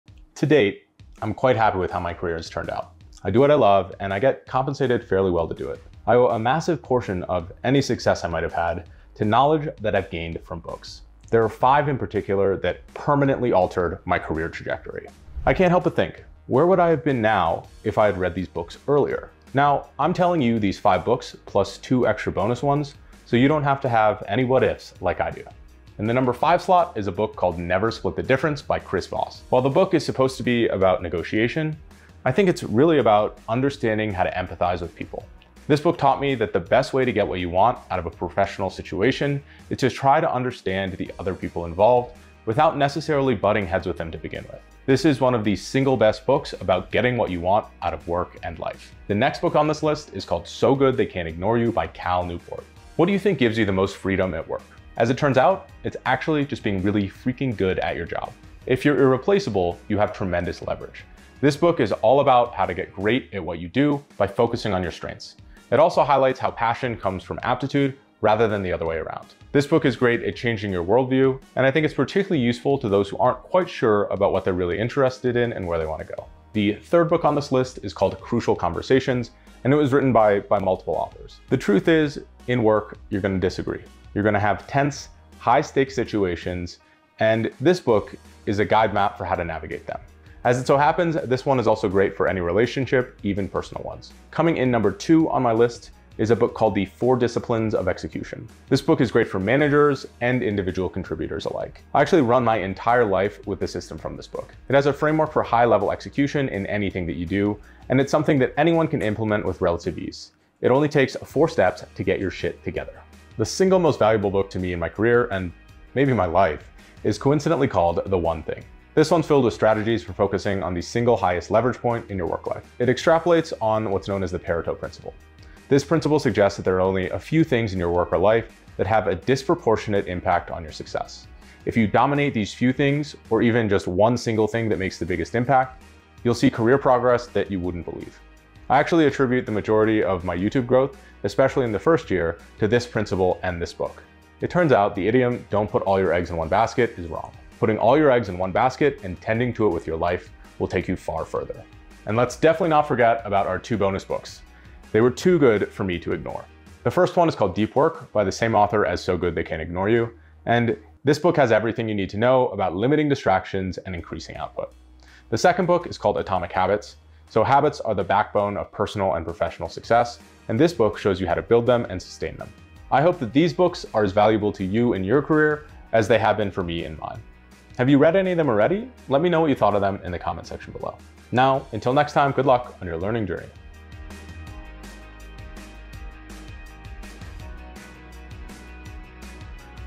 5 Books I Wish I Read Earlier In My Career
These 5 books changed the course of my career. I wish I had read them earlier! I'm telling you about them now so you can get ahead.

0:00 Intro
0:25 Book 1
0:49 Book 2
1:17 Book 3
1:34 Book 4
1:55 Book 5
2:31 Bonus Books!

Thanks to @TinaHuang1 & @EcZachly_ for the broll and thumbnail help!

Never Split the Difference
So Good They Can't Ignore You
Crucial Conversations
The One Thing